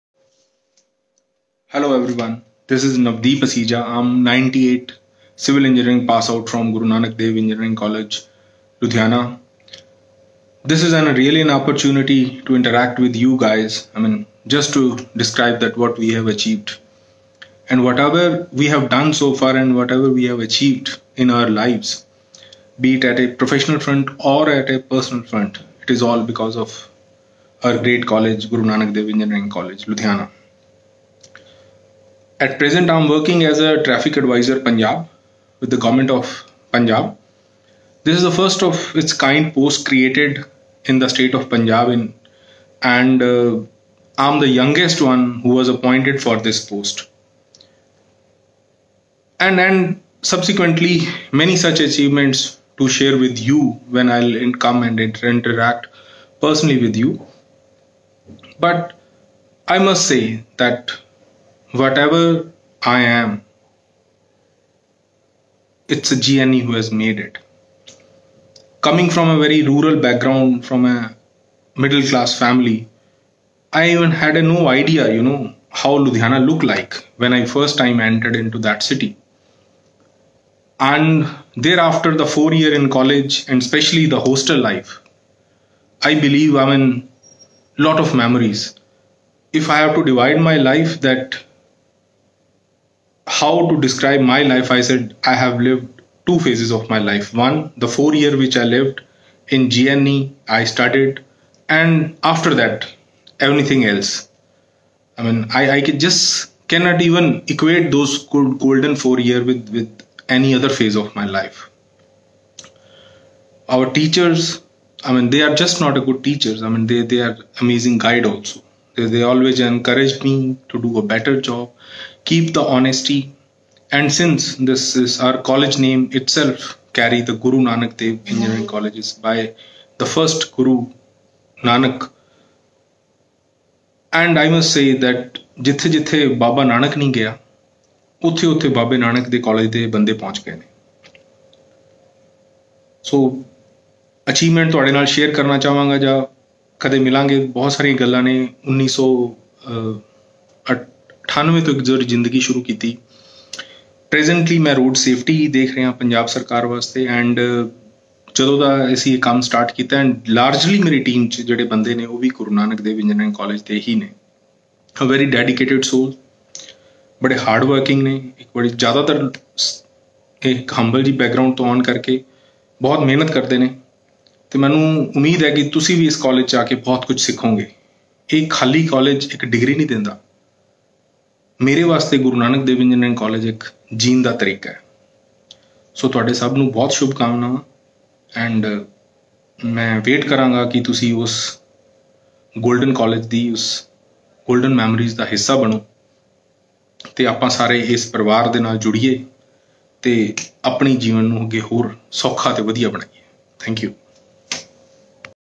GNDEC alumni, Dr.Navdeep Asija | Traffic advisor, Govt. of Punjab | 1998 Civil Engg. Graduate
GNDEC alumni, Dr.Navdeep Asija, Traffic advisor to Government of Punjab, Civil Engineering Graduate sharing his views and experience of the college and the immense contribution of the college towards his success.